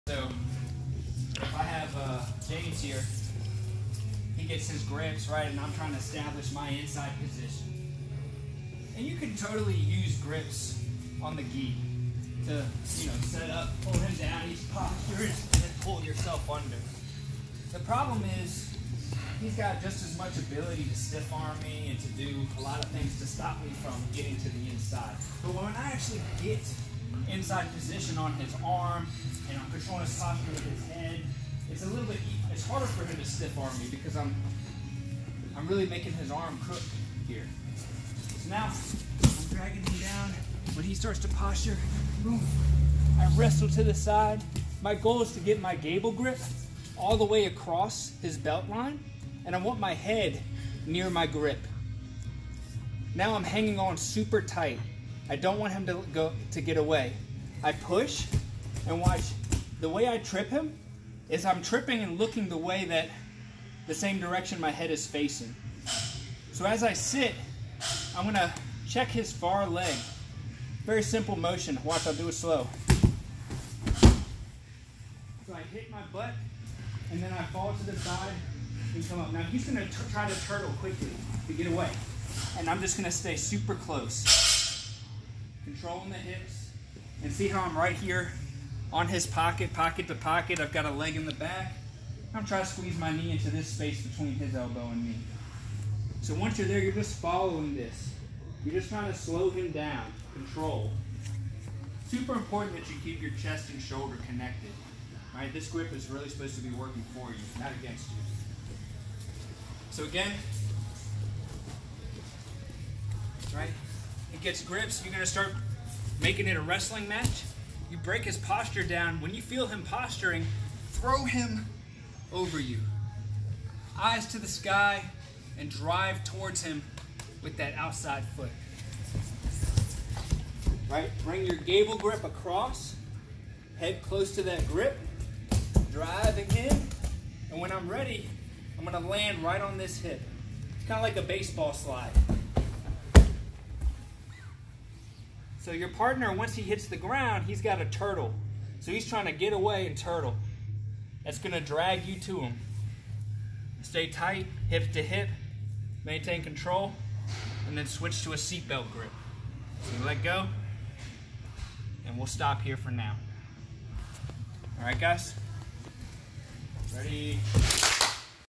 Duck Under to the Takedown - Profectus Jiu Jitsu Wilco
If you can get behind your opponent with the "Duck Under" ...you can take them down very easily with a baseball slide style trip! Translates well into nogi or gi sparring!